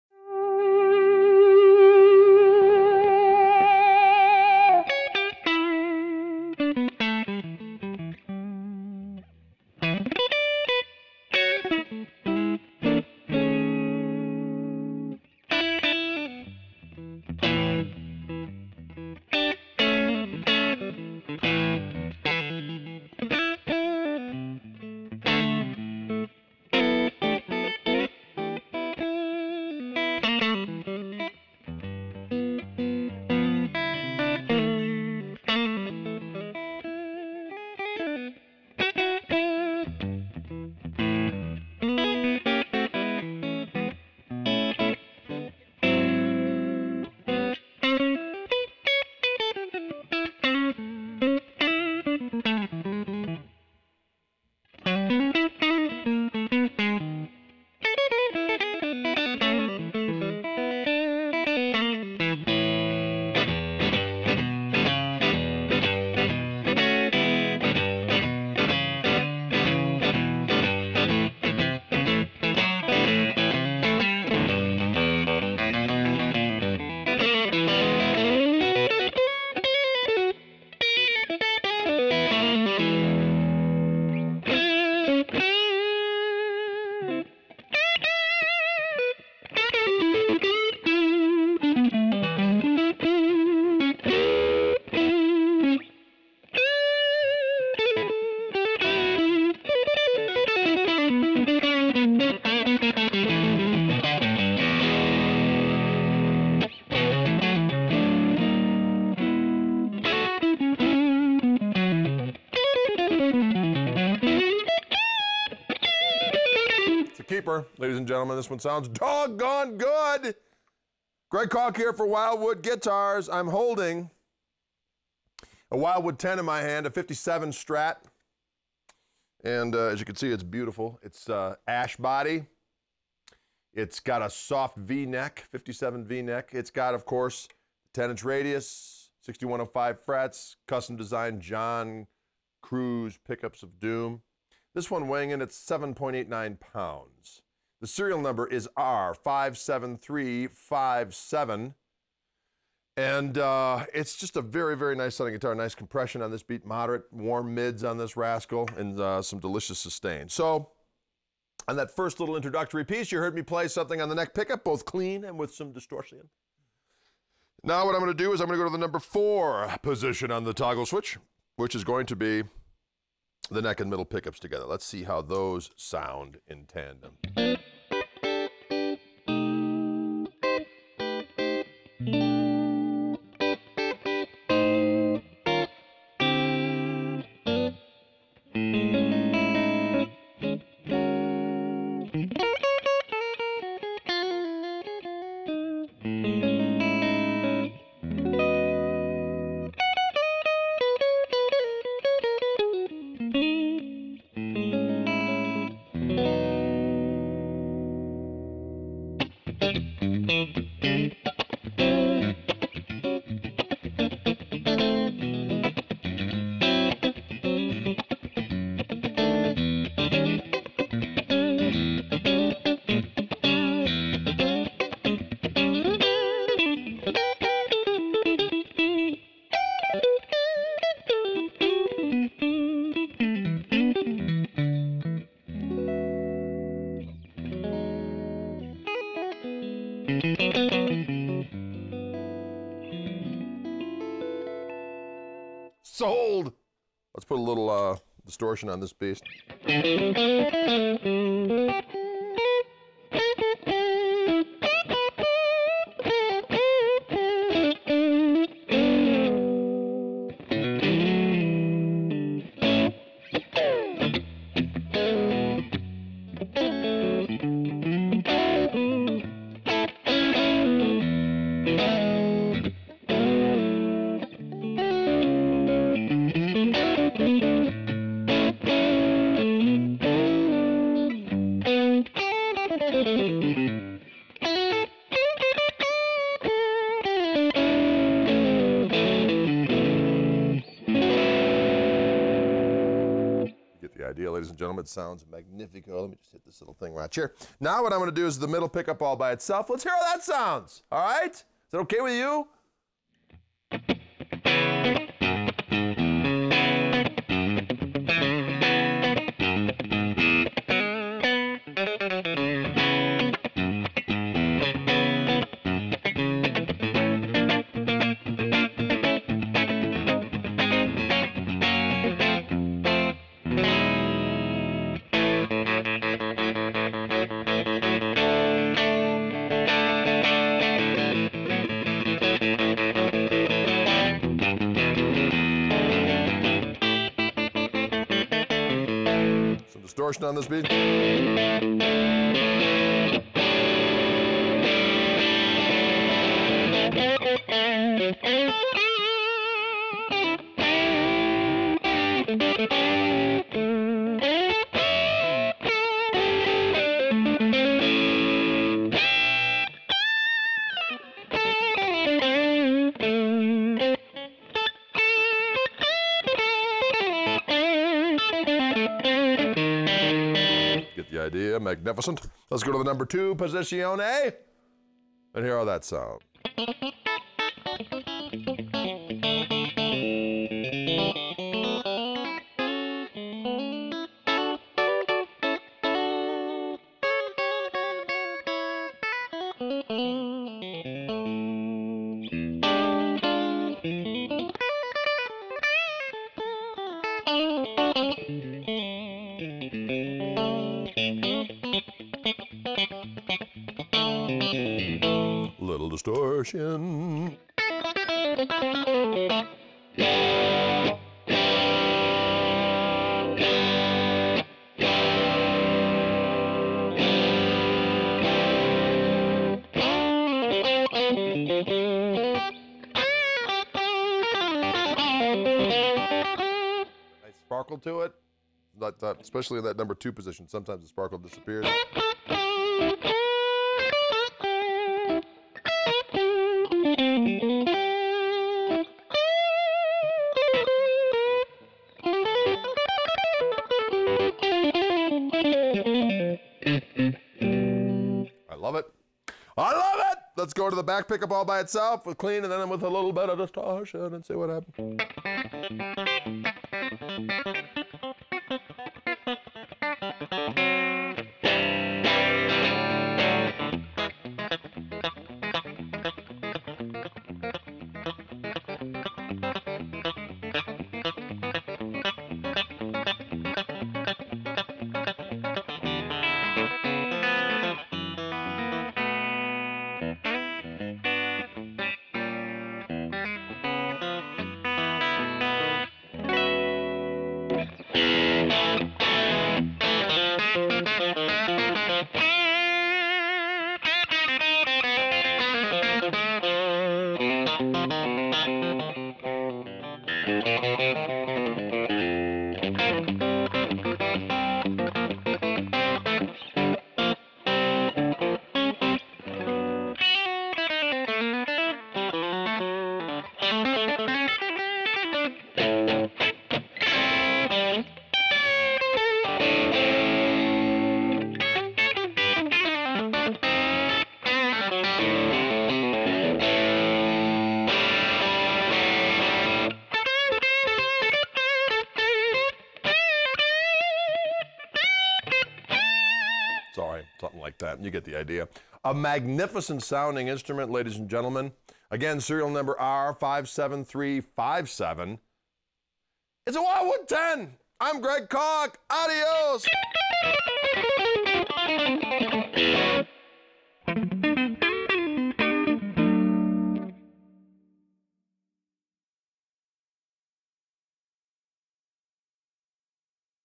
Fender Custom Shop Dealer Select Wildwood "10" 1957 Stratocaster Relic • SN: R57357
Straight from the Fender Custom Shop and Wildwood Guitars come some of the finest Strats and Teles we’ve ever had the pleasure of offering: the Fender Dealer Select Wildwood ""10s."" These instruments feature ""faded"" thin nitrocellulose finishes, hand-selected tone woods, Custom Shop pickups, and unique appointments like a 10"" fingerboard radius and 6105 frets. The guitars ship with a Certificate of Authenticity and a Custom Shop Limited Edition black hardshell case. The guitars combine modern features with vintage vibe and tone, and they're available exclusively at Wildwood Guitars.

Serial Number: R57357
Brand: Fender Custom Shop
Model: Dealer Select Wildwood "10" '57 Stratocaster
Type: Stratocaster
Year: 1957
Finish Color: Faded White Blonde
Aging: Relic
Finish Type: Nitrocellulose Lacquer
Weight: 7.89 lbs.
Body Wood: Ash
Neck Wood: Quarter Sawn Maple
Neck Shape: Mid 60's "C"
Fingerboard: Maple
Fingerboard Radius: Exclusive Wildwood Custom "10" Radius
Inlays: Clay Dot
Scale Length: 25.5"
Width at Nut: 1.650"
Nut Material: Bone
Frets: Dunlop 6105
Pickups: 3 John Cruz Limited Single-Coils
Controls: 1 Volume, 2 Tone, 5-Way Switch
Hardware : Nickel
Bridge: Vintage Tremolo
Tuners: Vintage Style with Nickel Buttons
Pickguard: Single-Ply Parchment
COA: Yes